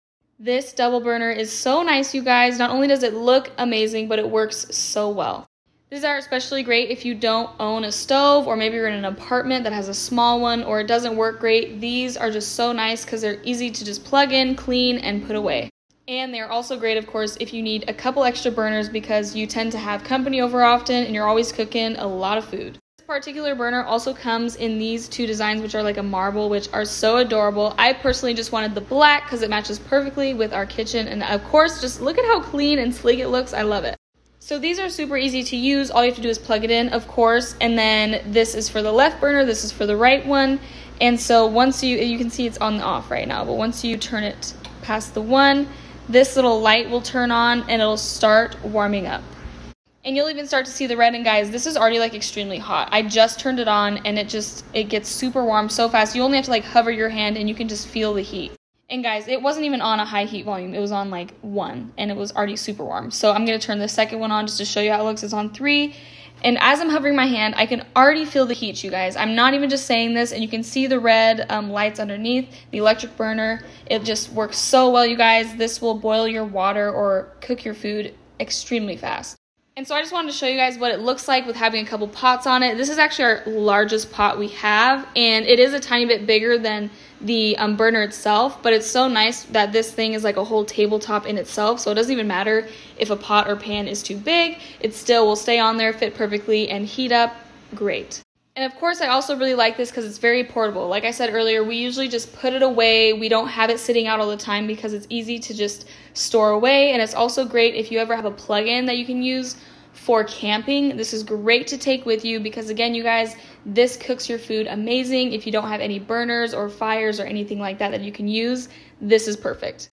CUSIMAX Double Burner - Amazon
This double burner is so great! So useful if you have people over a lot or you don't have a stove. These get hot in seconds and can hold large pots and pans. Super easy to use and very effective!